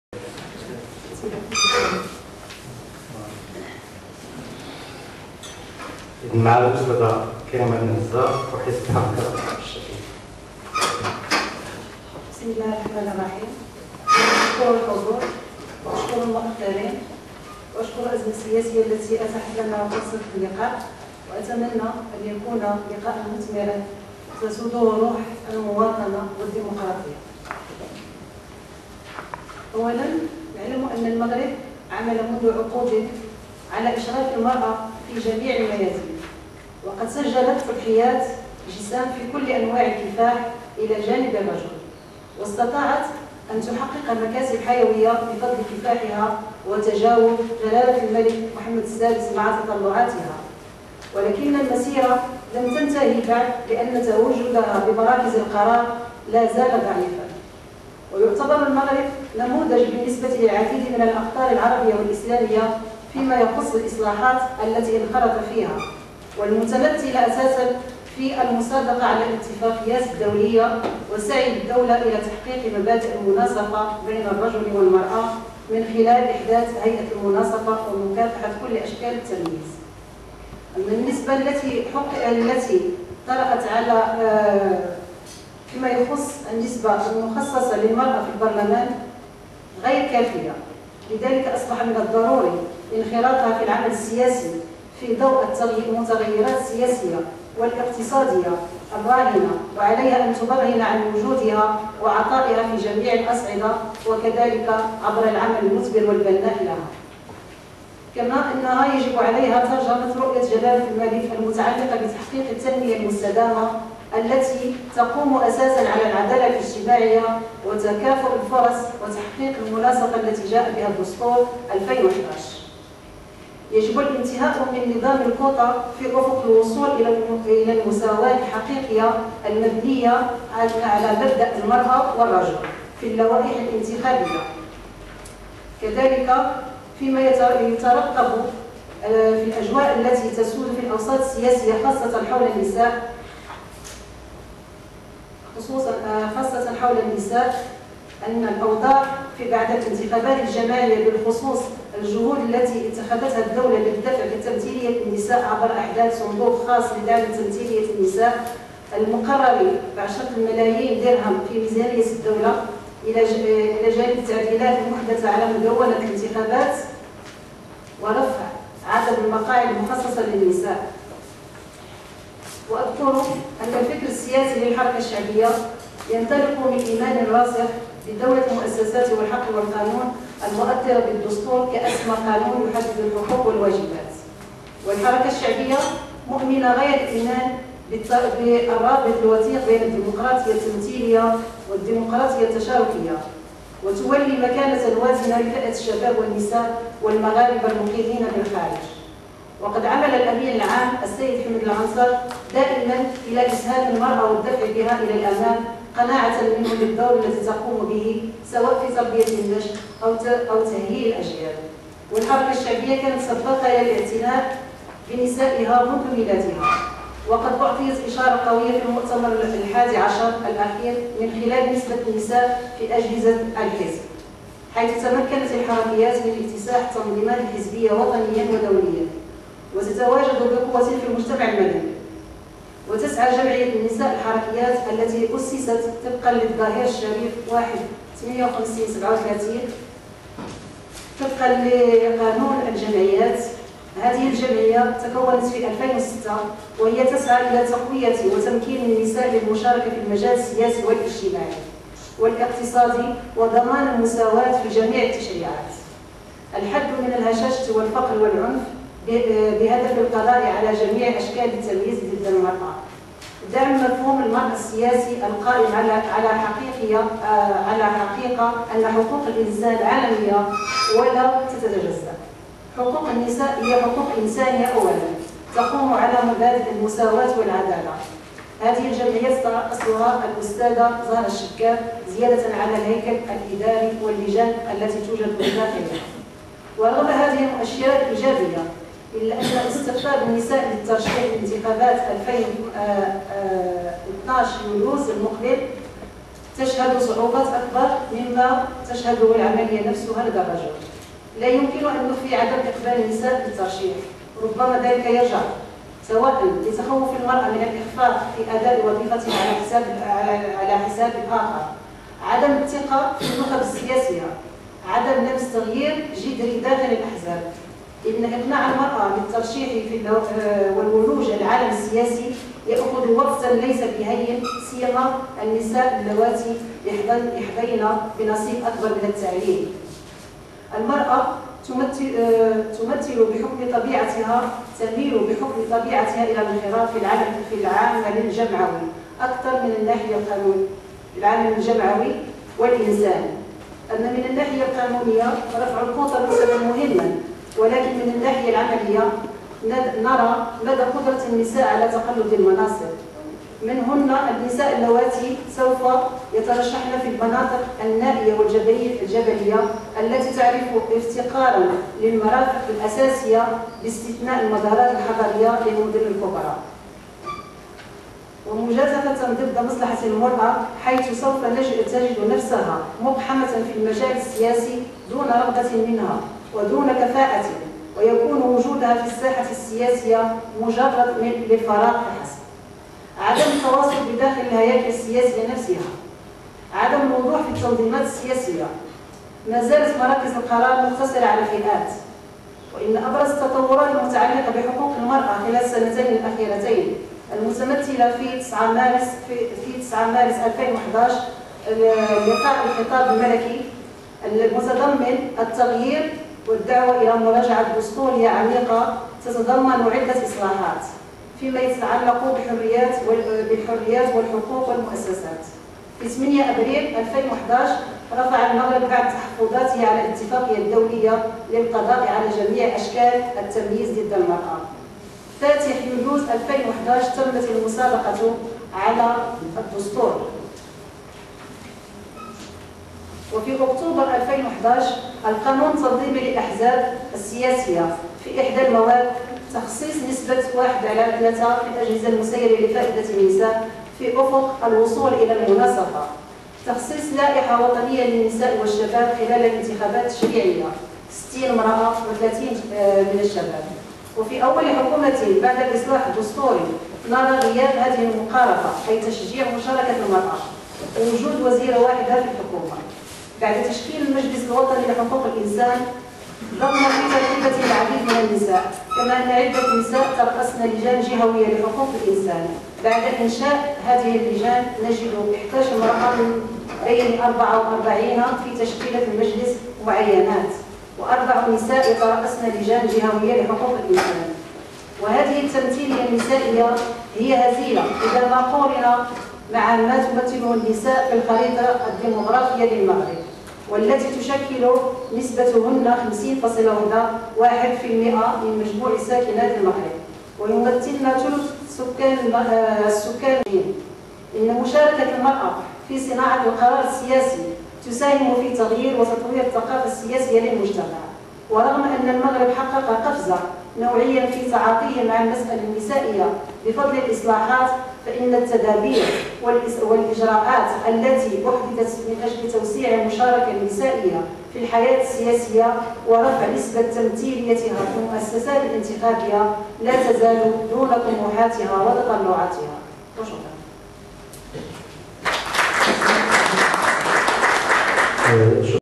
تدخل كريمة بن عزي في ندوة المرأة والسياسة
تدخل كريمة بن عزي في ندوة المرأة ورهانات العمل السياسي